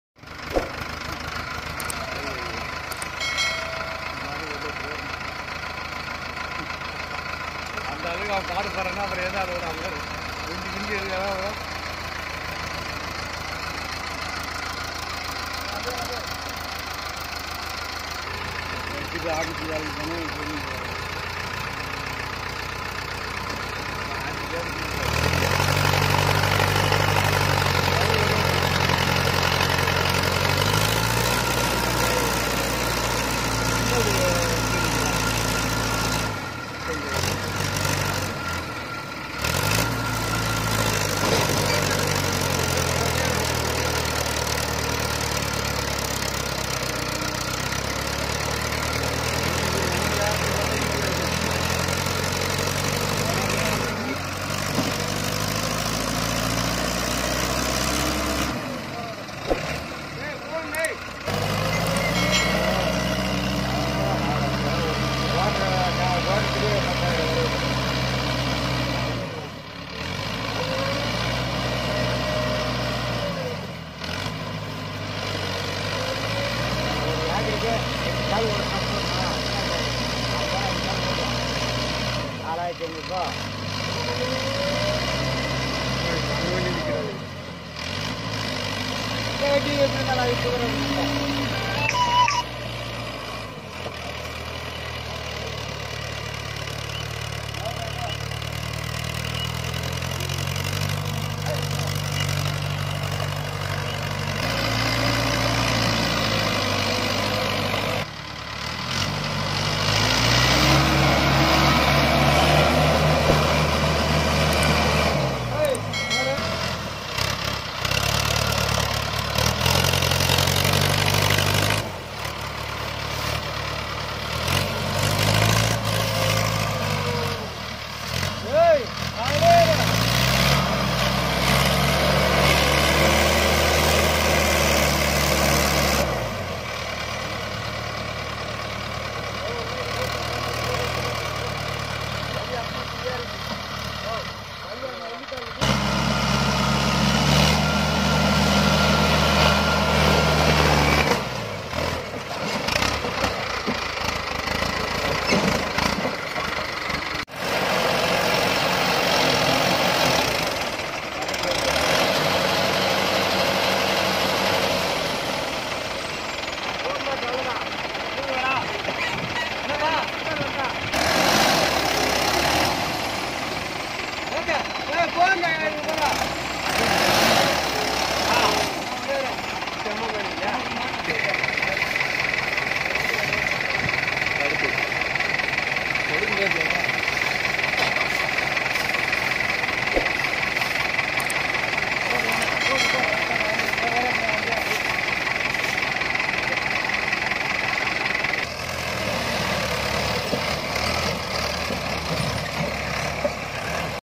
Mahindra 595 Di tractor stuck in mud remove Mahindra Arjun555 ultra power pulling | tractor video
Mahindra 575 Di tractor stuck in mud remove Mahindra Arjun555 ultra power pulling | tractor video come to my village